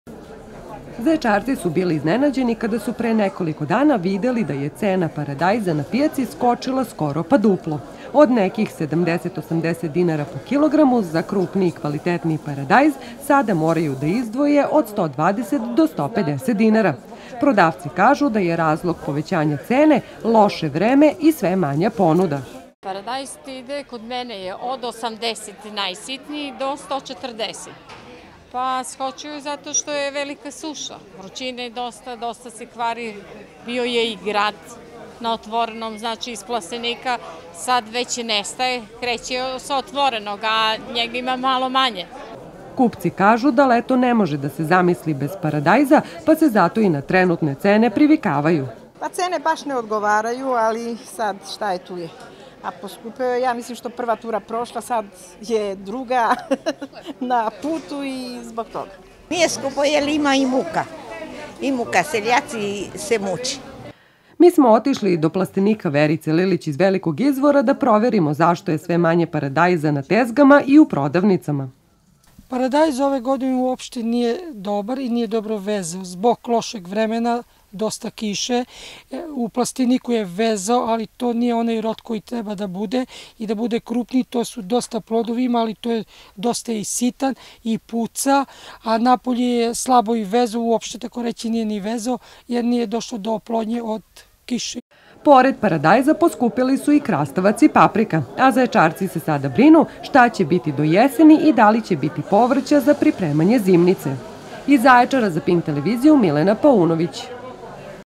Drastično skočile cene paradajza
Kiša, grad, ali i velike vrućine doveli su do toga da cena paradajza na Zaječarskoj pijaci i u prodavnicama drastično skoči. Proizvođači kažu, da potražnje ima, ali da je ponuda slaba, jer su strukovi paradajza u plastenicima i na otvorenom slabo rodili kao i da je kvalitet paradajza loš.